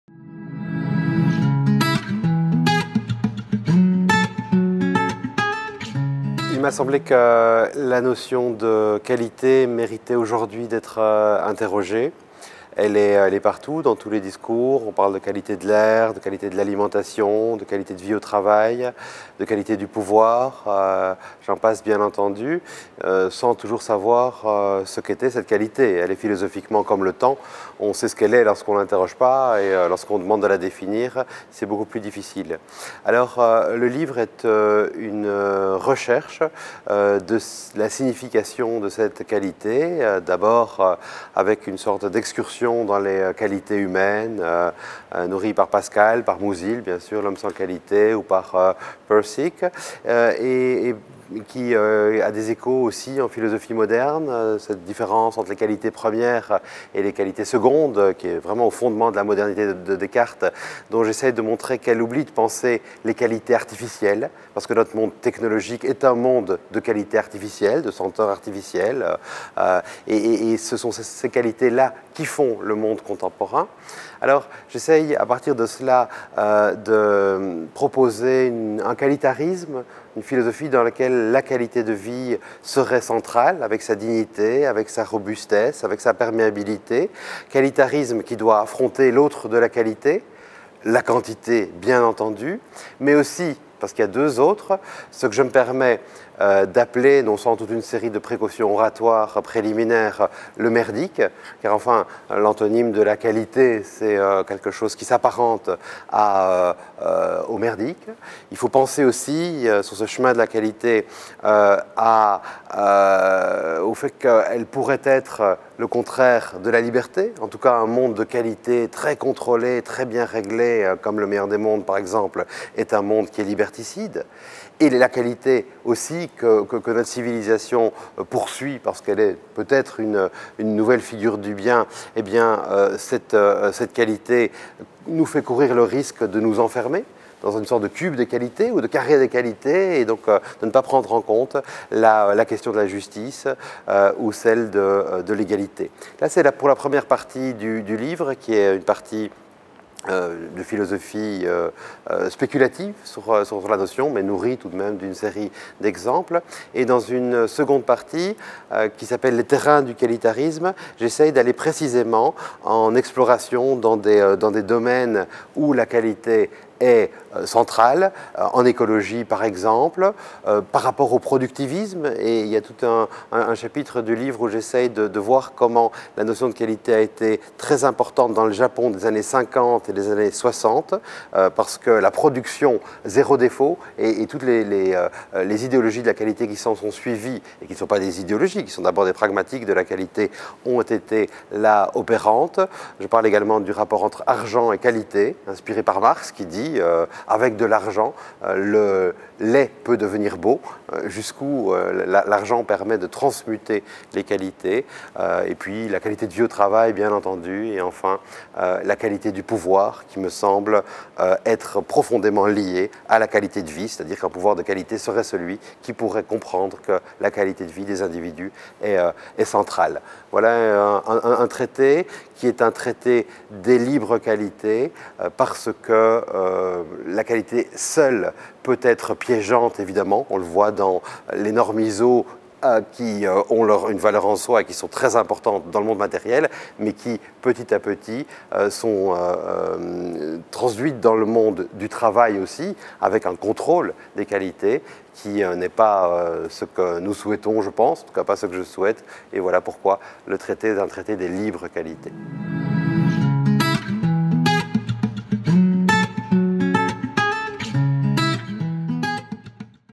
Pascal Chabot - Traité des libres qualités
Pascal Chabot vous présente son ouvrage "Traité des libres qualités" aux éditions PUF. Rentrée Sciences humaines Septembre 2019.